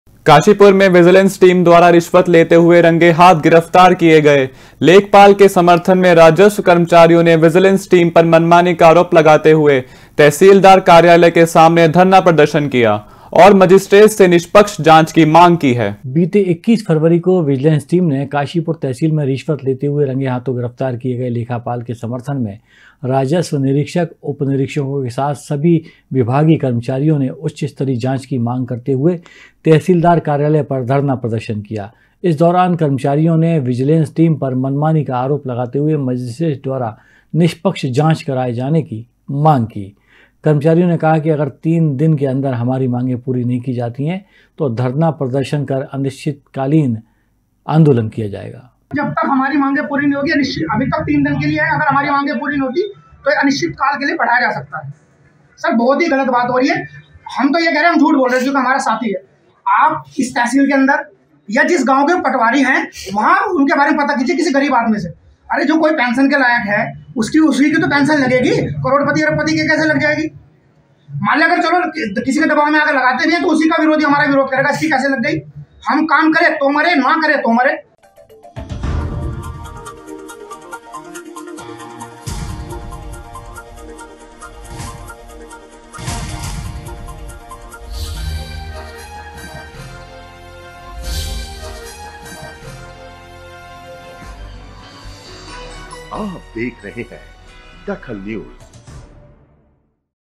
DAKHAL NEWS विजिलेंस टीम के खिलाफ धरने पर बैठे राजस्व कर्मचारी
विजिलेंस टीम के खिलाफ धरने पर बैठे राजस्व कर्मचारी
कर्मचारियों ने मजिस्ट्रेट द्वारा निष्पक्ष जांच की मांग की